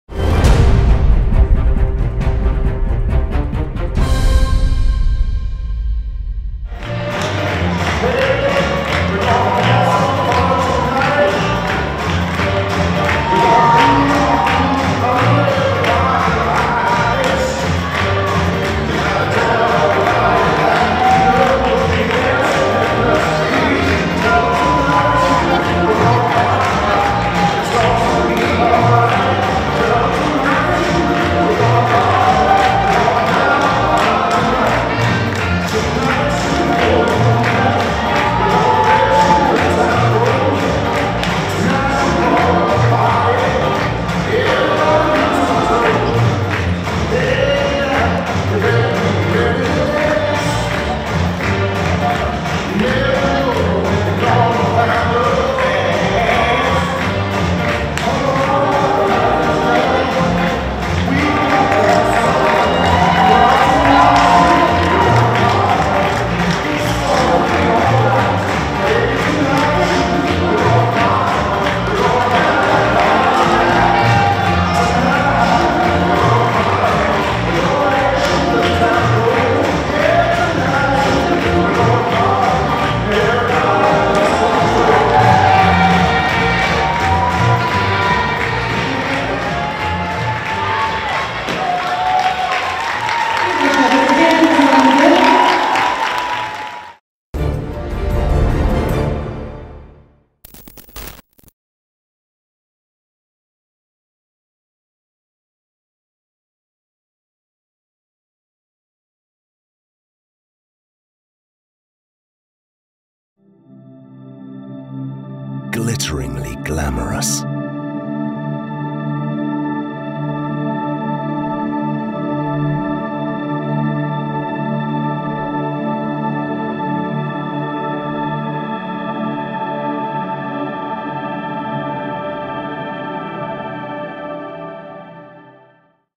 Bures - Khadjeh-Nouri, GER | 2015 GS LAT Moscow | R4 J | DanceSport Total
Marek Bures - Anastasiia Iermolenko Khadjeh-Nouri, GER, dance the Jive in Round 4 of the 2015 WDSF GrandSlam Latin on 24 October in Moscow, RUS.